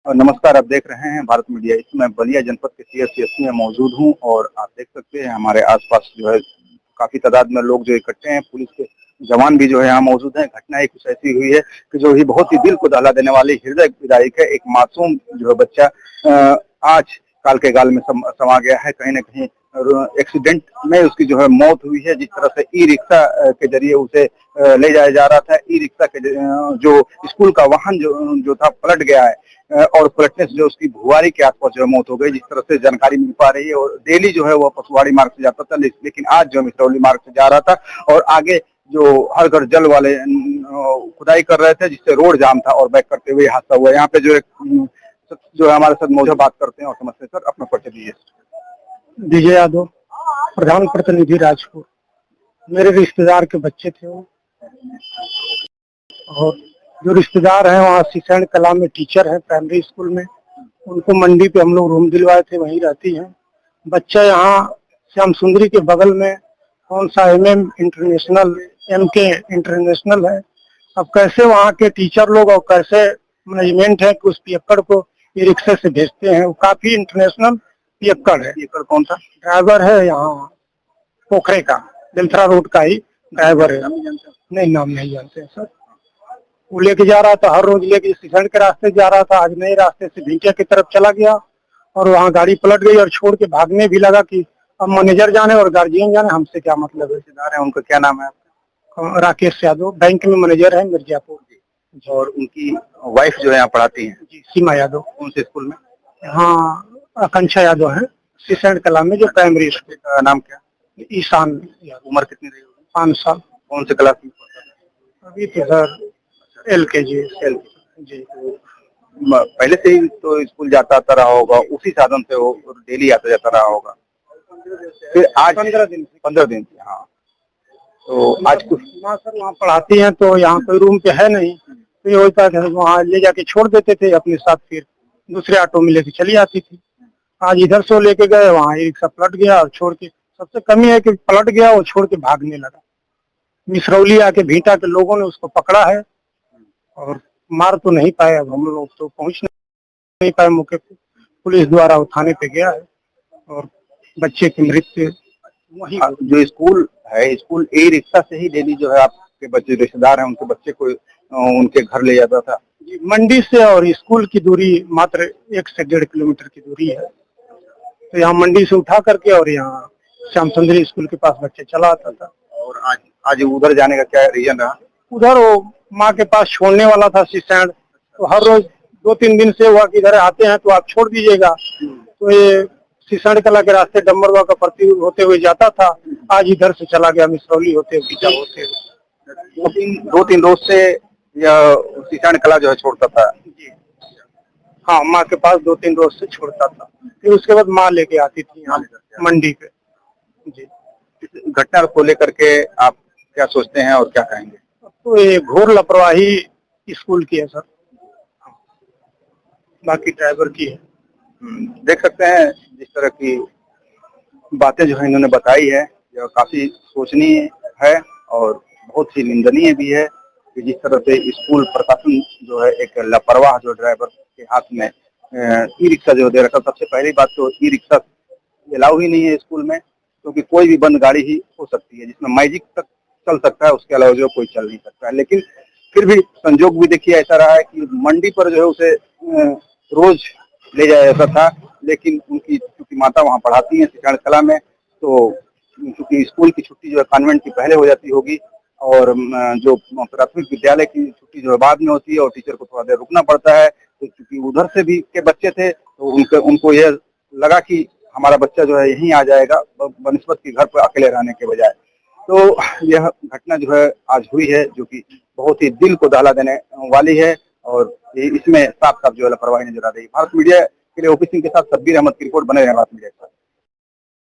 मासूम छात्र की मौत मे ई-रिक्शा चालक की लापरवाही आई सामने
स्कूली बच्चों के ई-रिक्शा ड्राइवर को बताया इंटरनेशनल शराबी